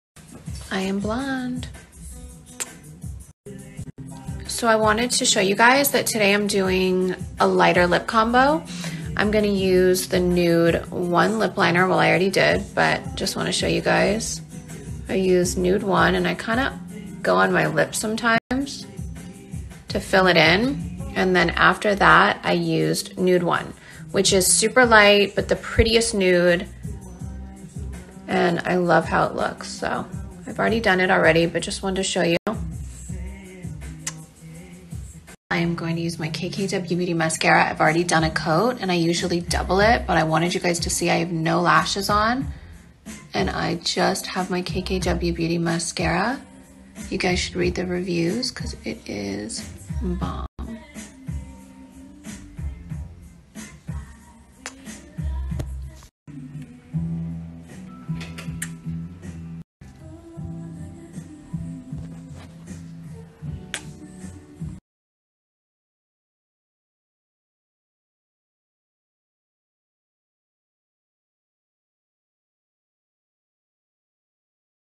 Kim Kardashian Doing her Makeup with KKW Beauty
Kim doing her nude makeup.